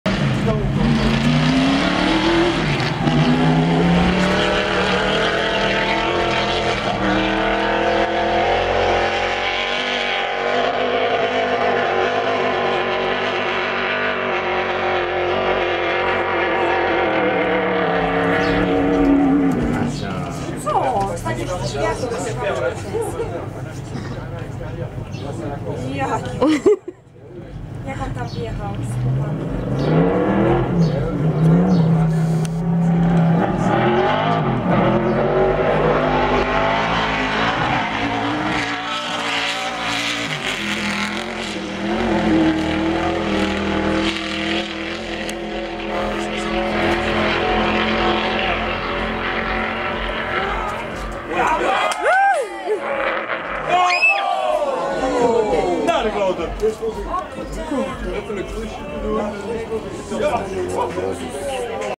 Rekord Świata-Guinnessa w długości skoku samochodem Tignes
Rekord Świata w długości skoku samochodem w Tignes - Alpy Francuskie, a właściwie próba pobicia tego rekordu przez kierowcę rajdowego Guerlain Chicherit , gdyż podczas skoku doszło do wypadku, auto żle wylądowało i kilkakrotnie koziołkowało. Kierowca został przewieziony do szpitala ale na szczęście nic powarznego mu się nie stało...więcej na